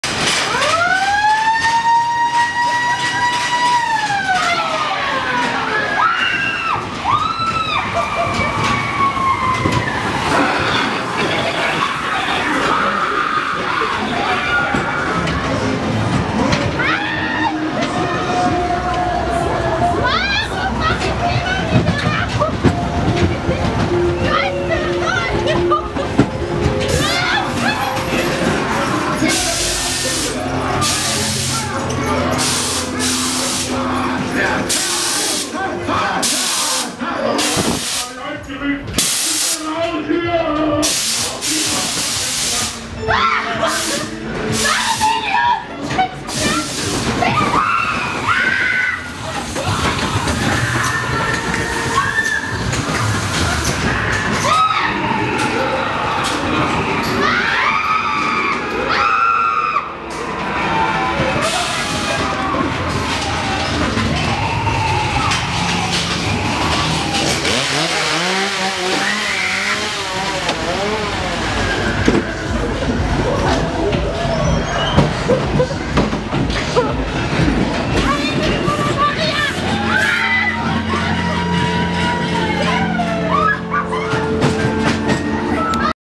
Fahrt mit der Geisterbahn "Haunted Castle" auf dem Fuldaer Schützenfest 2024
Onride Video einer Fahrt mit der Geisterbahn "Haunted Castle" auf dem Fuldaer Schützenfest am Abend des 2. August 2024.